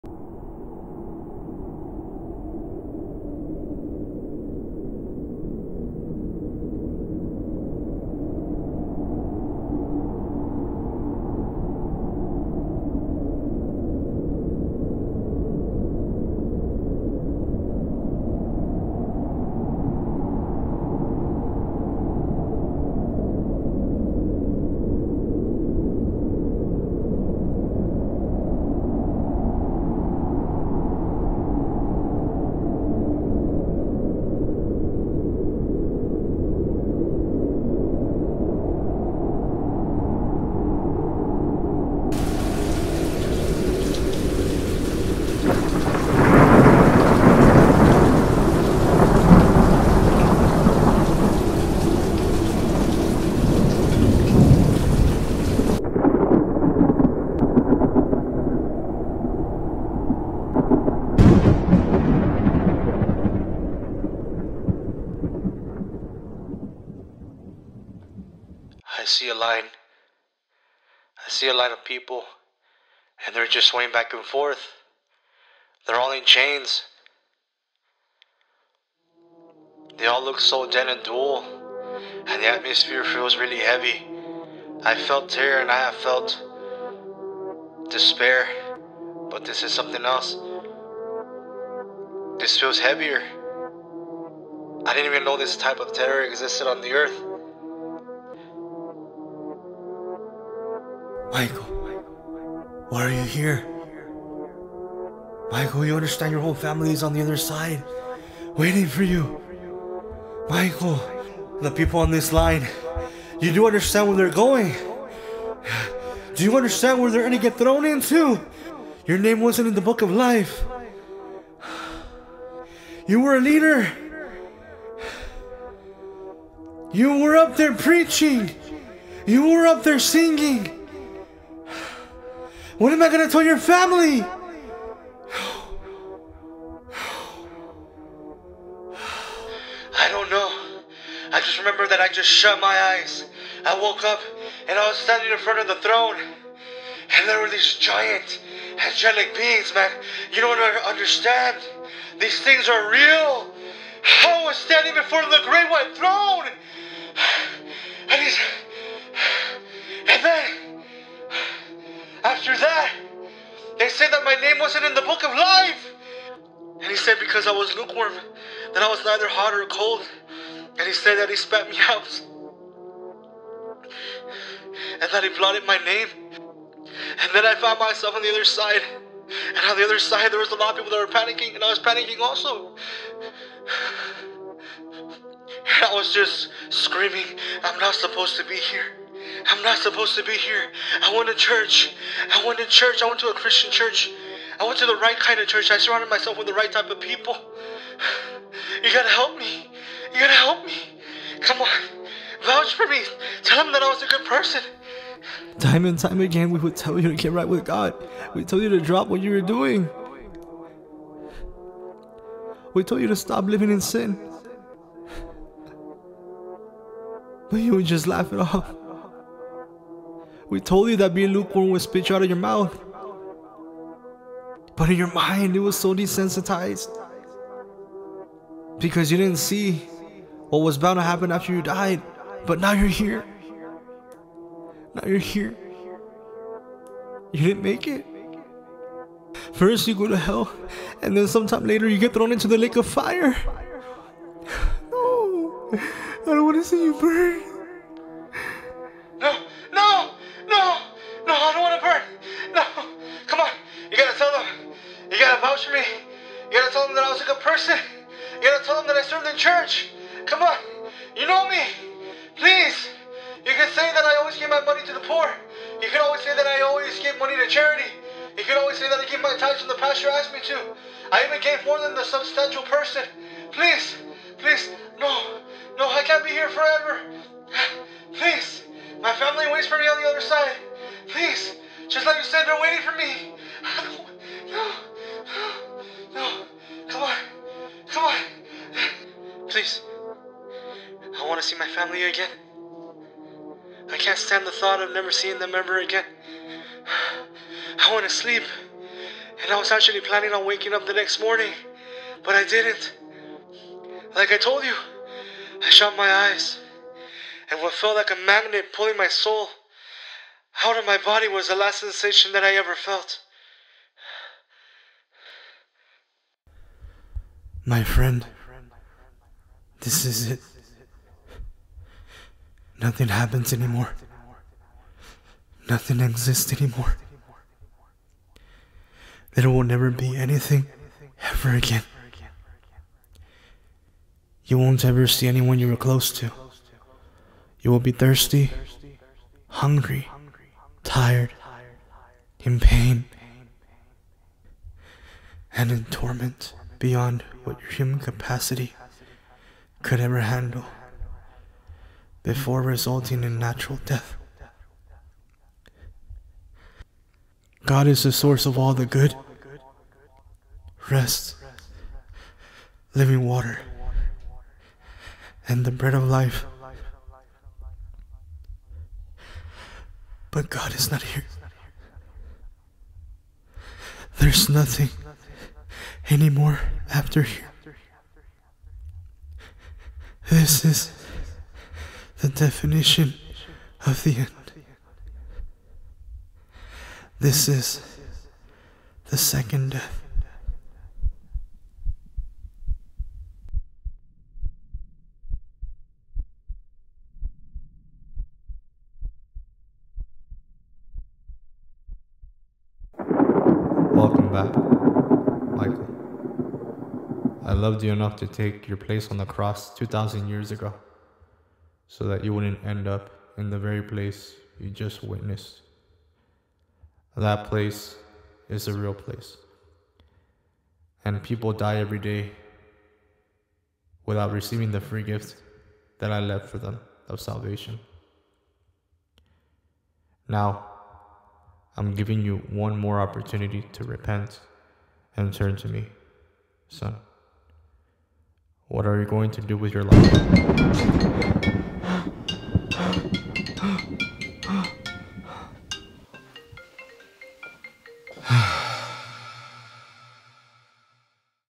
The Horrors Of Death
Hell Heaven podcast This is the reality of what happens when someone dies without Christ. Jesus tasted death for all of us, so you wouldn't have to.